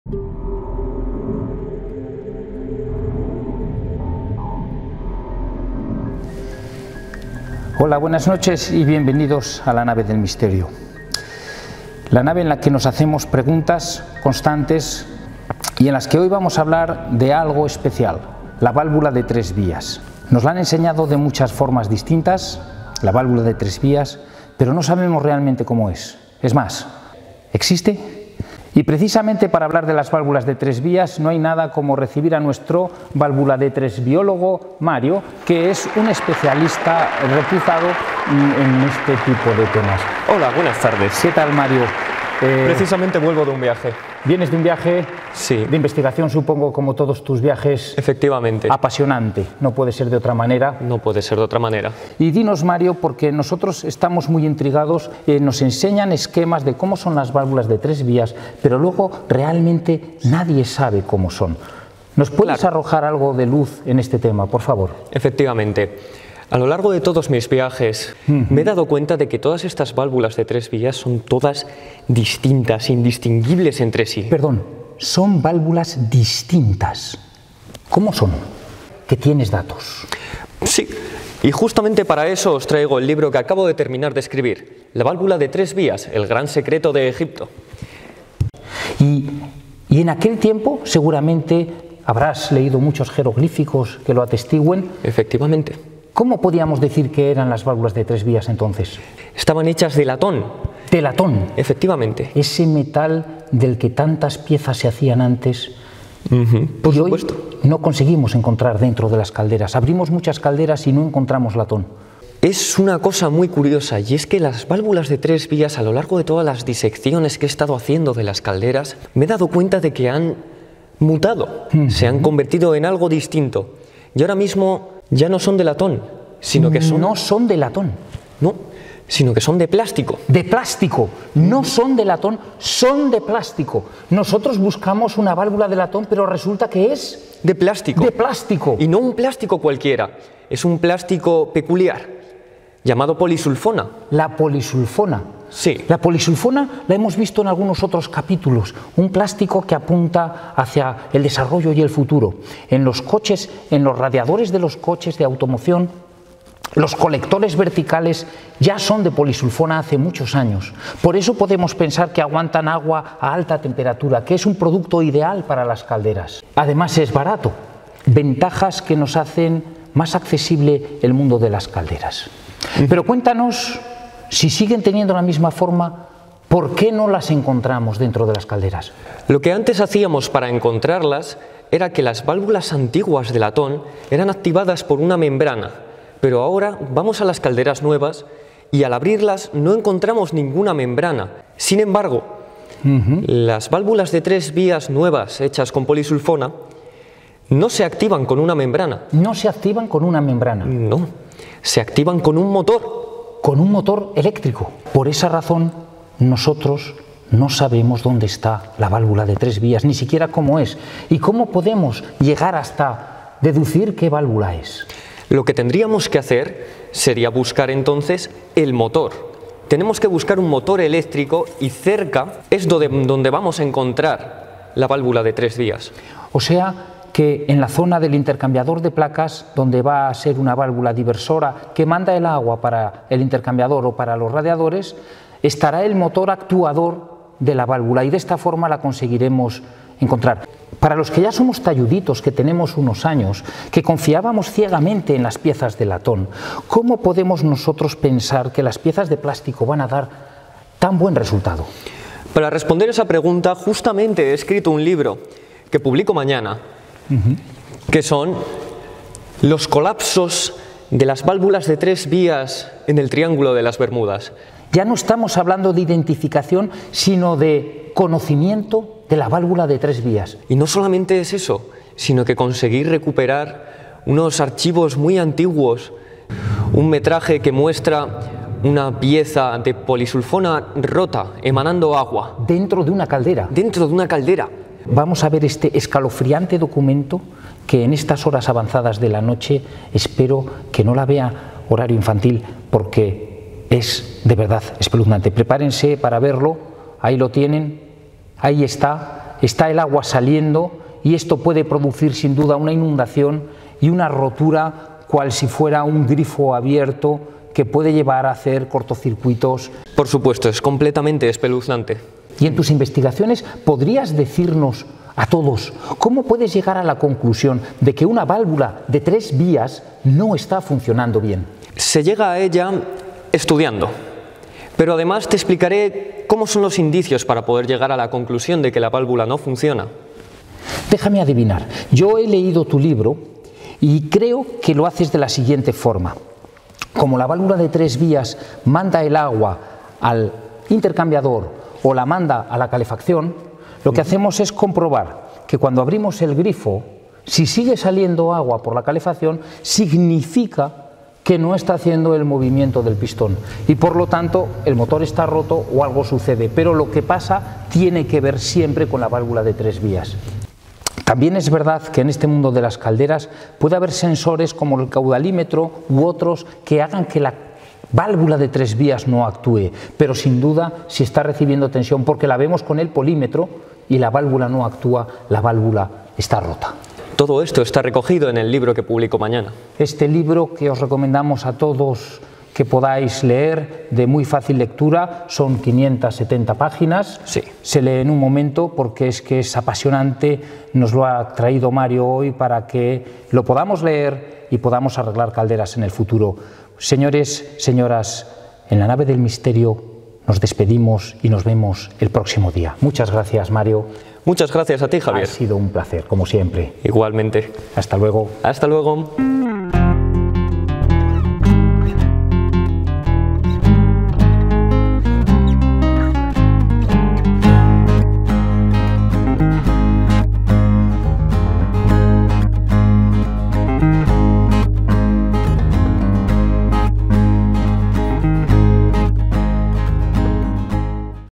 Cómo localizar la Válvula de 3 vías en caldera mural. Explicado y fácil.
Este vídeo se publica en la noche de Halloween, donde reina el misterio.

IMPORTANTE: No todas las calderas tienen válvulas de 3 vías. Recordar que existen las calderas bitérmicas, y que con las calderas de condensación pueden encontrar la perfecta ocasión para resurgir. Jojo JOjo joooo

Este vídeo como todos los demás, tiene un objetivo de difusión de ideas y nunca como tutorial, ya que para hacer un tutorial es muy importante saber a quién se tutoriza, sus destrezas y capacidades.

Agradecimiento muy especial a Mario del Pico por su disponibilidad y compañía.

Este vídeo es parte de un curso y como tal debe entenderse.